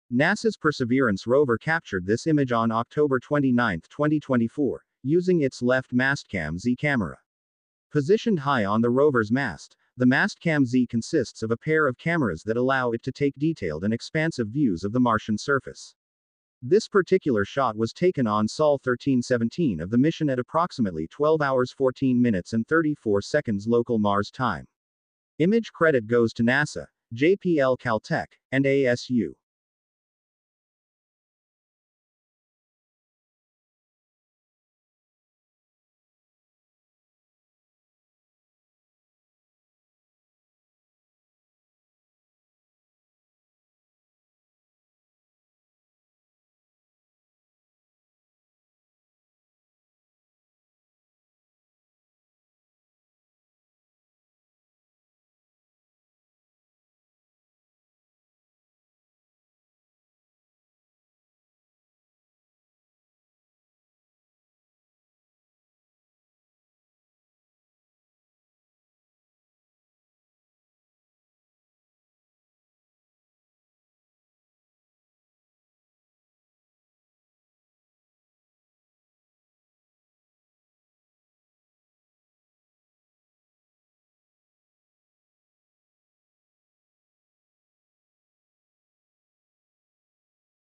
Nasa Unveils Breathtaking 4k Video Footage||New Video Footage of Mars
NASA has released stunning 4K video footage showcasing the mesmerizing landscapes of Mars. The captivating visuals, courtesy of NASA/JPL-Caltech/ASU/MSSS, provide a breathtaking glimpse into the Red Planet's unique terrain.